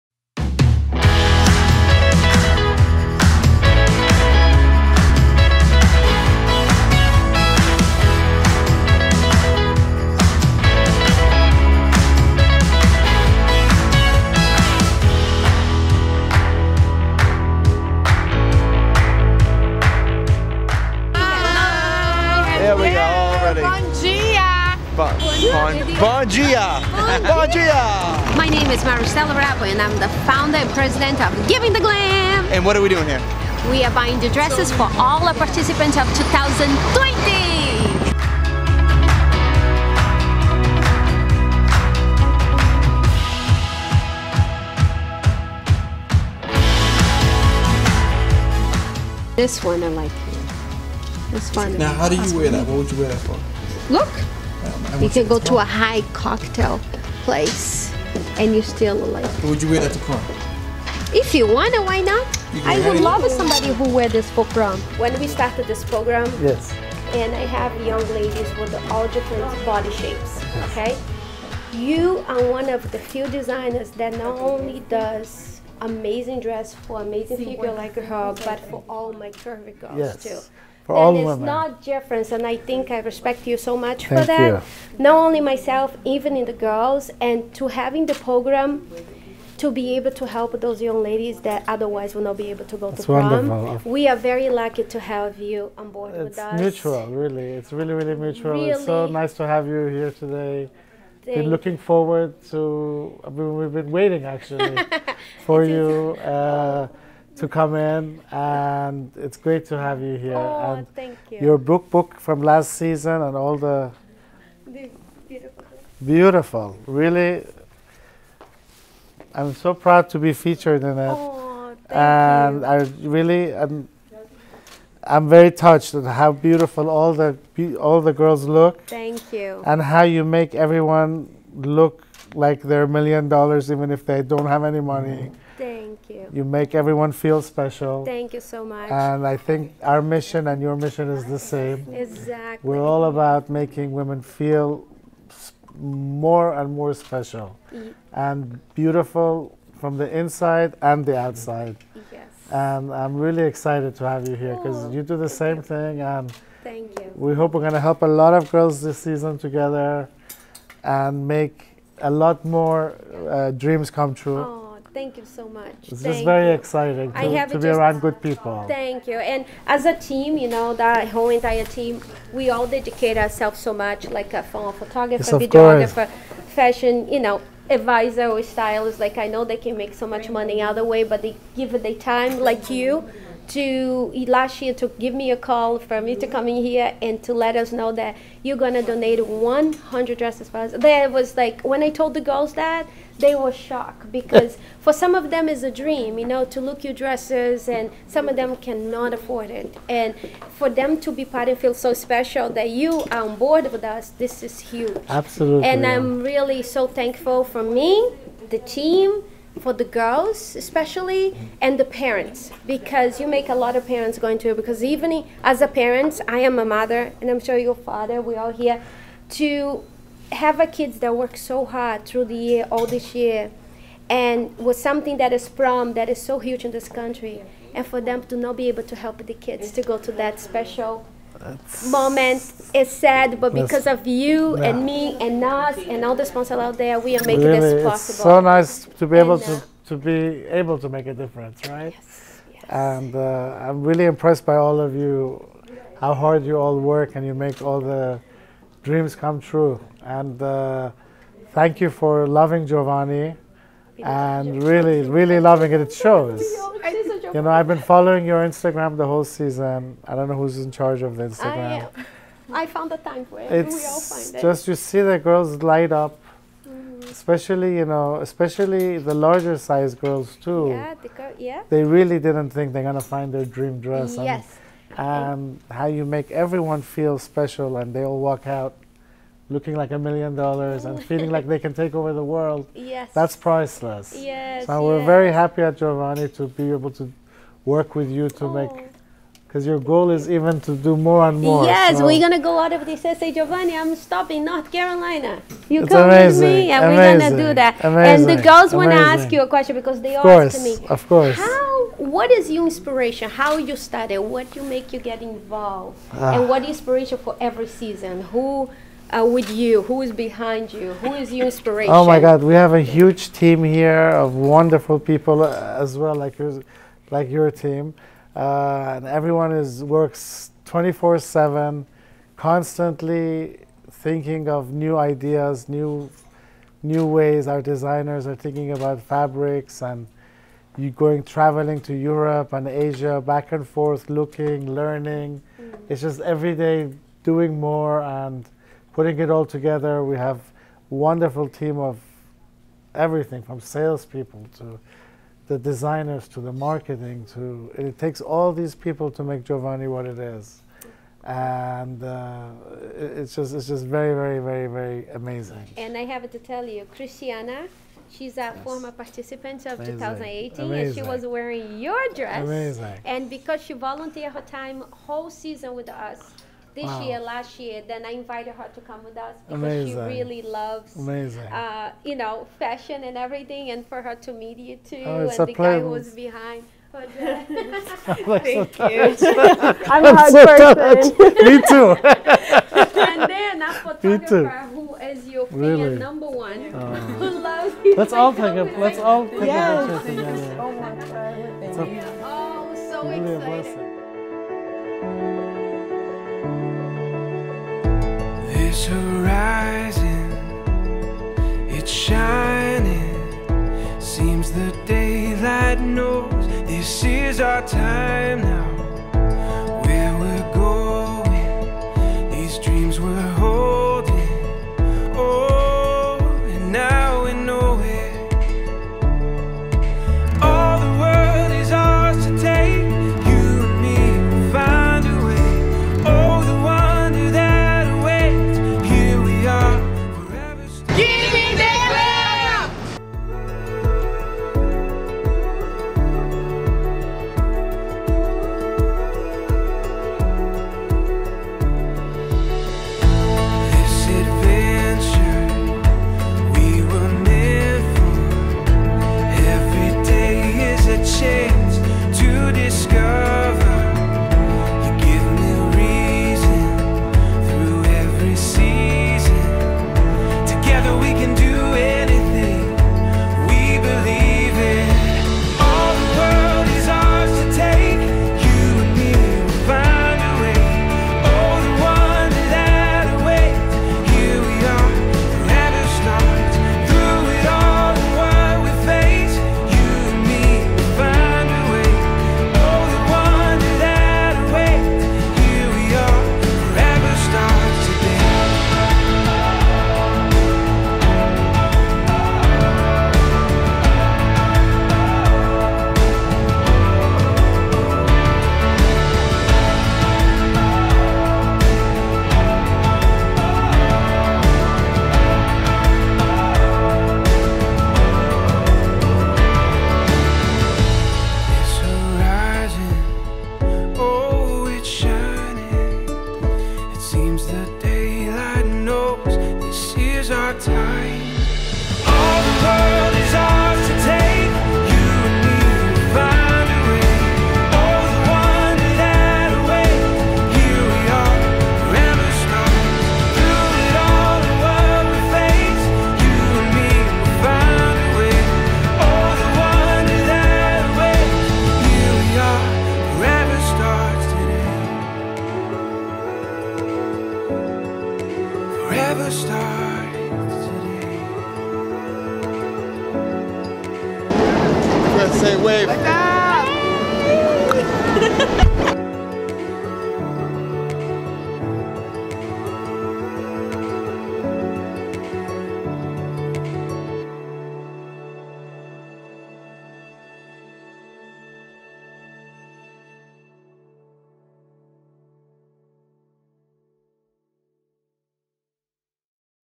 Giving the Glam picking out the Fabulous 2020 Jovani gowns at Jovani showroom in New York
The men behind Jovani Fashion, Abraham Maslavi who alongside his brother Saul Maslavi have continued to make their father's dreams into a reality. They make every woman from size 00 to size 24 feel fabulous and special! Abraham and the Jovani family have given our participants the chance to feel special! Thank you so much for your kindness, we are forever grateful and so happy to have Jovani family on board with us! ❤👗💙 Jovani Fashion
Videographer  JLC production
Photography by Studio Art photo

O homen por trás do Jovani Fashion, Abraham Maslavi, que, ao lado de seu irmão Saul Maslavi, continuaram a transformar o sonho do pai em realidade. Eles fazem todas as mulheres, do tamanho 00 ao tamanho 24, se sentirem fabulosas e especiais! Abraão e a família Jovani deram as nossas participantes a chance de se sentirem lindas e especiais! Muito obrigado por sua gentileza, somos eternamente gratos e muito felizes por ter a família Jovani conosco!